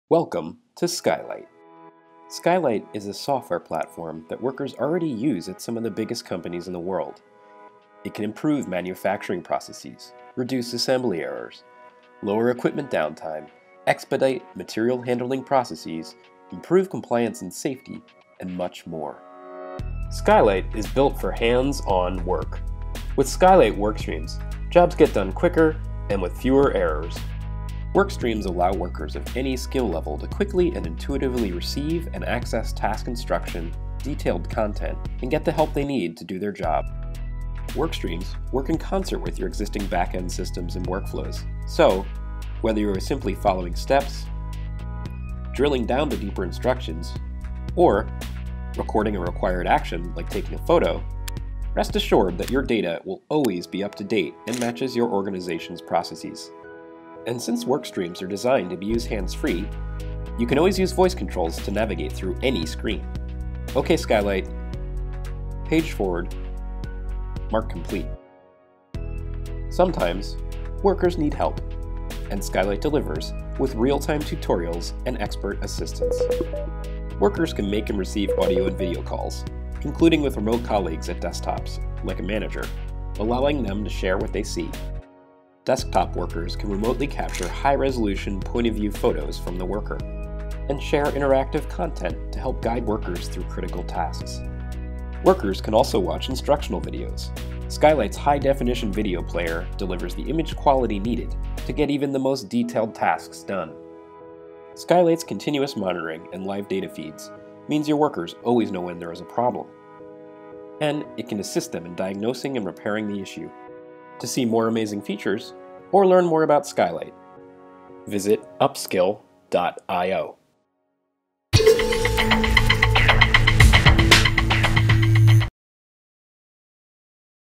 Skylight: Explained in Two Minutes
Our technology platform, Skylight, powers smart glasses and other wearable devices for use in tasks across manufacturing, field services and logistics to help companies measurably and immediately improve quality, productivity, safety, workforce utilization and compliance.